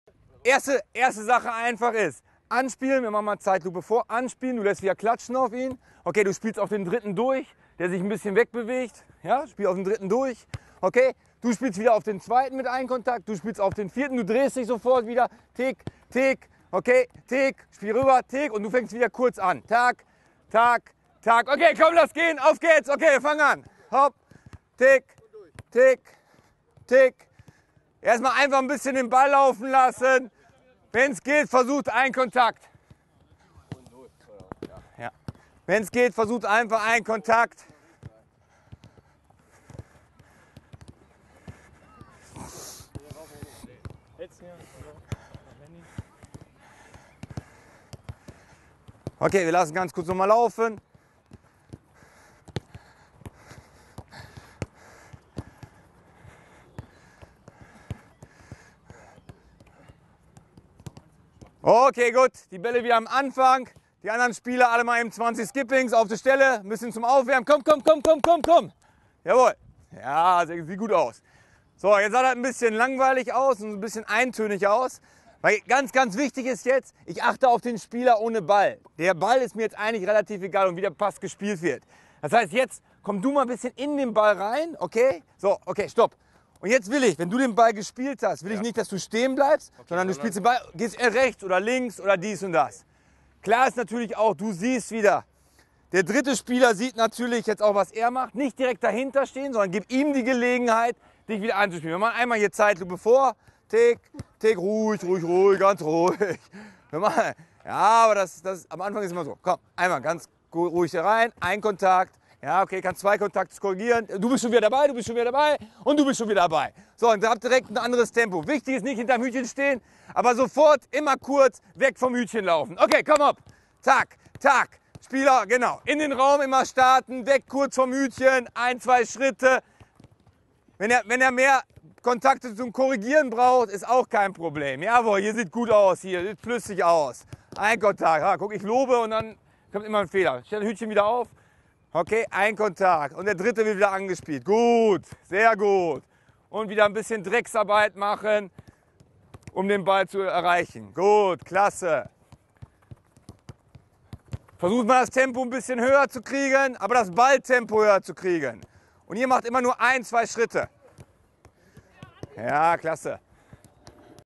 Fussballtraining mit Peter Hyballa - 20er-Gitter (1)
Die Grundform im 20er-Gitter. Mehr Infos zum Seminar

Inhalte Seminar 1:
    1. Tiqui-Taca, One Touch, Totaalvoetbal, vertikales Spiel – anders oder doch ähnlich?
    2. Drei Formen des Flachpasses: Totaalvoetbal - One touch – Tiqui taca
    3. Methodik – Trainingsformen richtig coachen
    4. Das 20er-Gitter – Coaching ist unbedingt notwendig.
    5. Vor- und Nachteile des Flachpassspiels
    6. Pass-Kombis im Warming-up
    7. Warum Flachpass?
    8. Passen en trappen - Positionsspiele - Pass-Partei-Spiele
    9. Under pressure – Alles unter Druck spielen
    10. Standardisierte Flachpässe